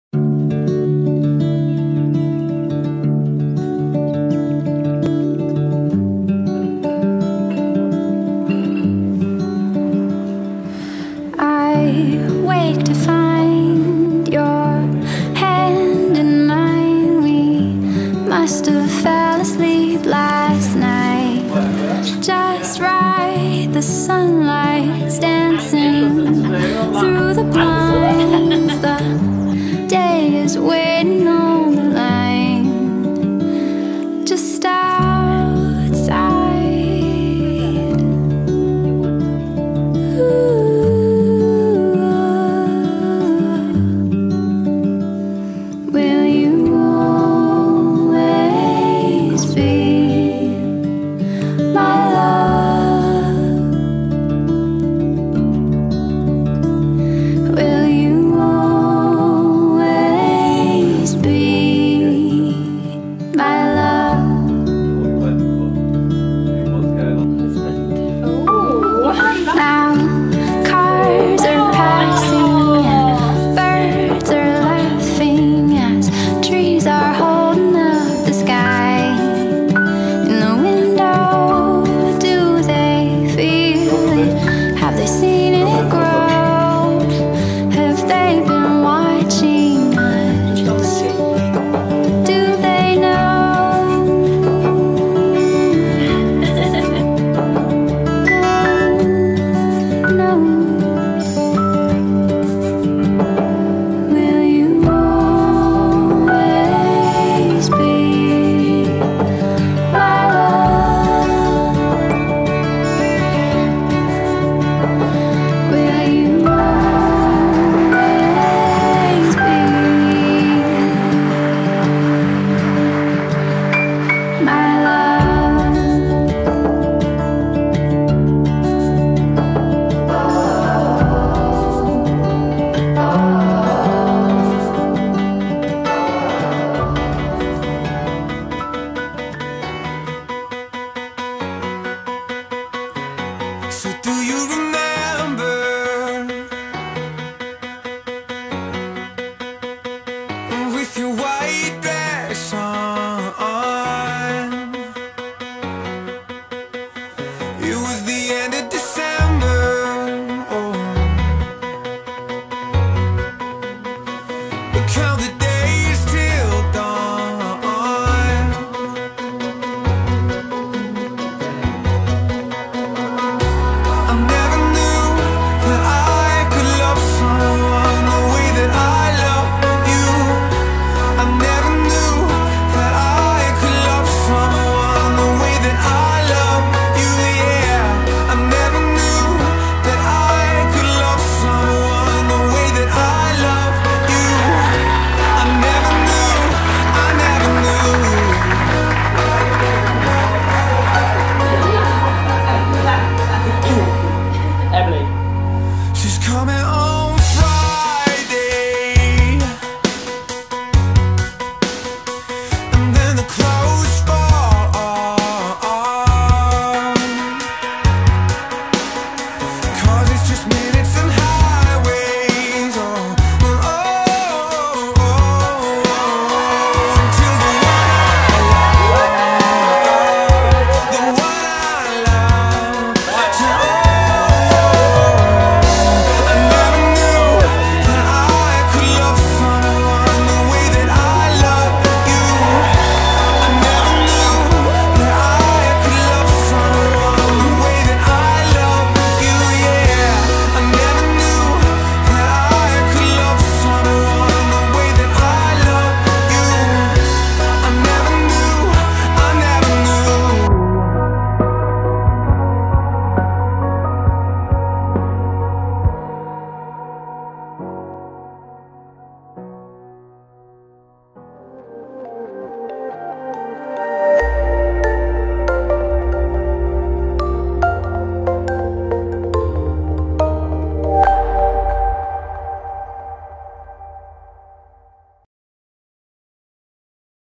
Kelham House - Emily + Ashley Wedding Film
Emily and Ashley had such a beautiful wedding day at the lovely St. Giles church in Balderton and then Kelham House in Newark.
The amount of effort that went into every detail made the day so unique to them, it was absolutely stunning.

We hope you enjoy this Highlights Film.